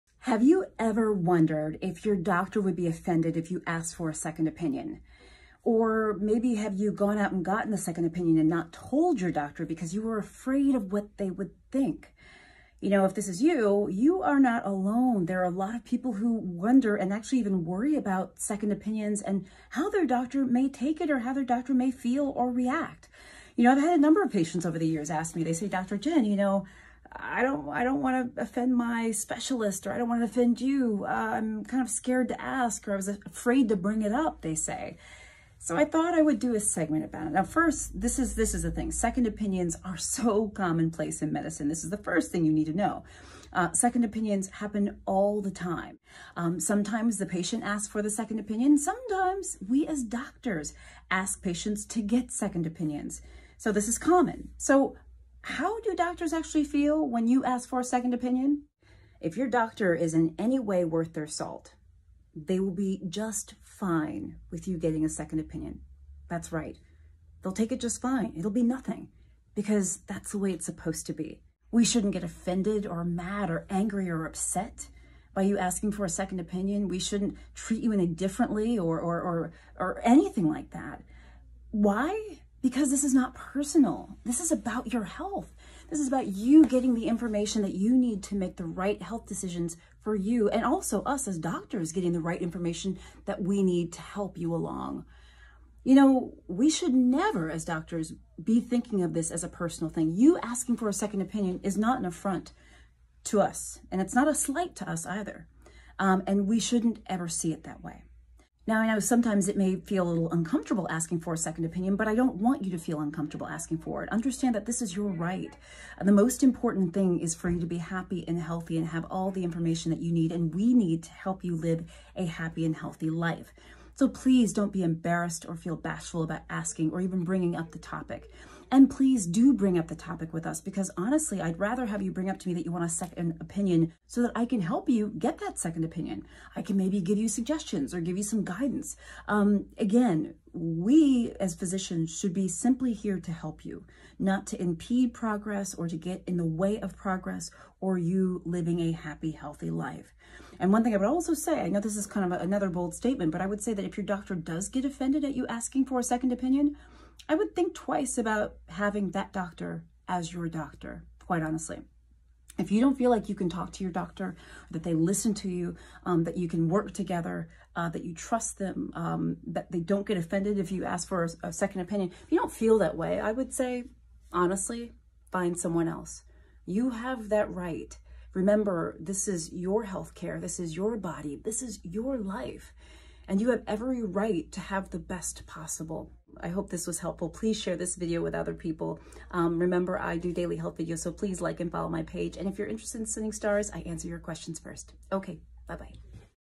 Will Your DOCTOR BE OFFENDED if You Ask for a SECOND OPINION? When Should You Get a Second Opinion?
Will Your Doctor Be Offended if You Ask for a Second Opinion? When Should You Get a Second Opinion? I discuss the answers in this video.